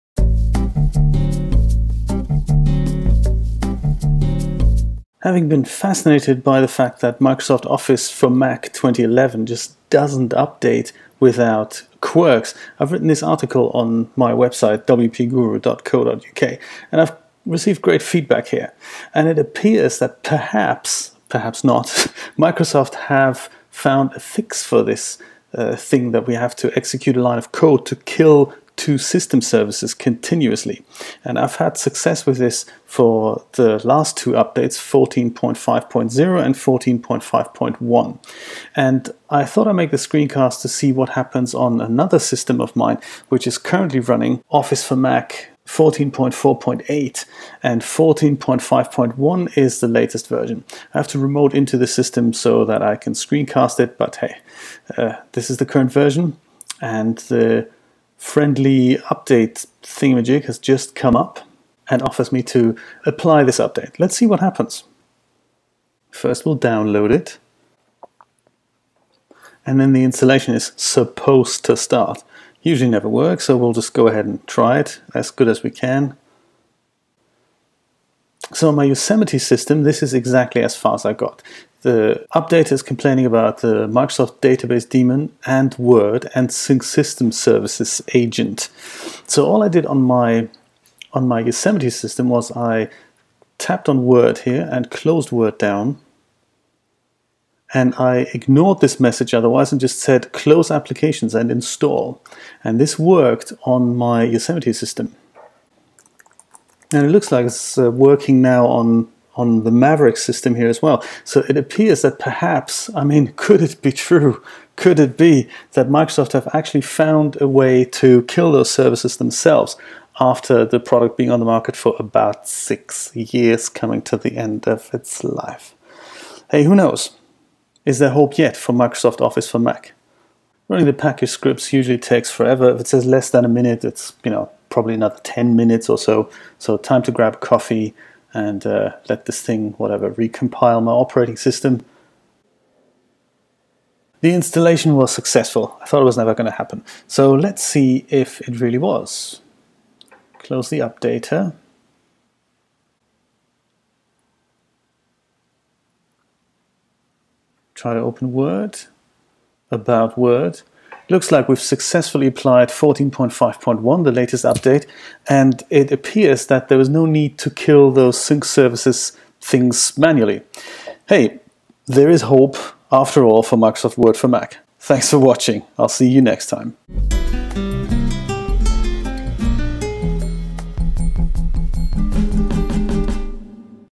Microsoft Office For Mac 2011 Update
A while ago I've written an article that explains how to update Office for Mac 2011 despite the two system services hindering us from doing so

As of Office for Mac 14.5.0, this "backdoor patch" appears no longer necessary. In this video I'm showing how I could successfully update the product without intervention.

Could it be that Microsoft have found a way to make this happen 6 years after the product's release?